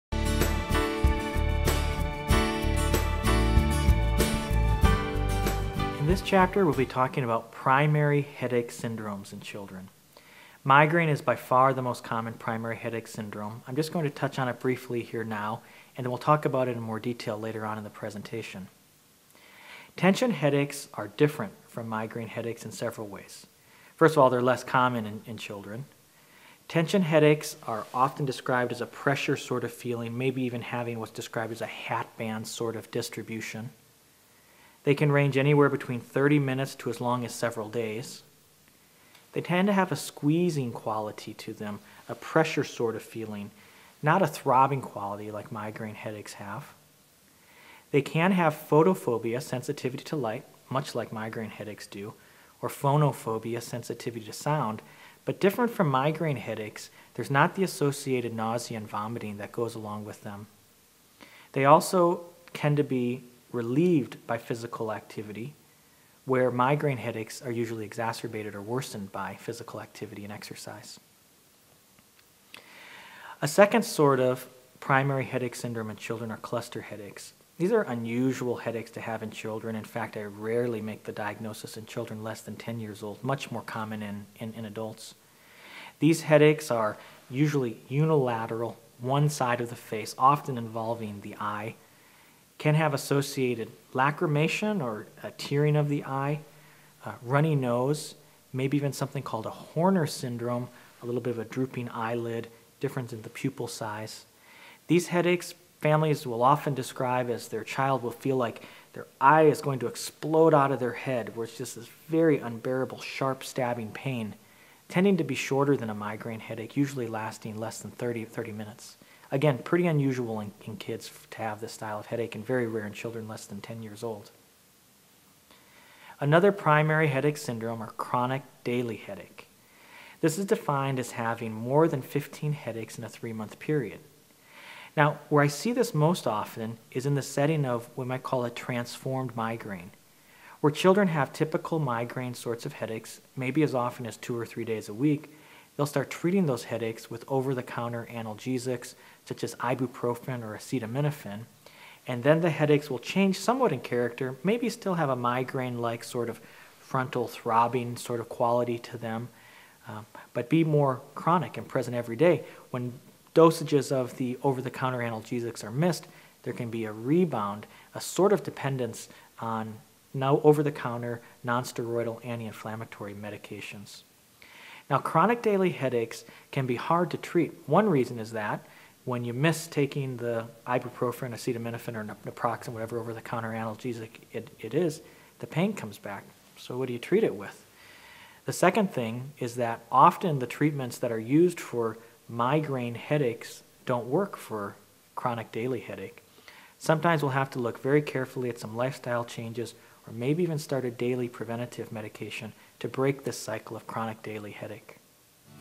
Headache 03 Headache Syndromes
Dr Andrew McIntosh  MD, a board-certified pediatric neurologist, discusses primary childhood headache syndromes.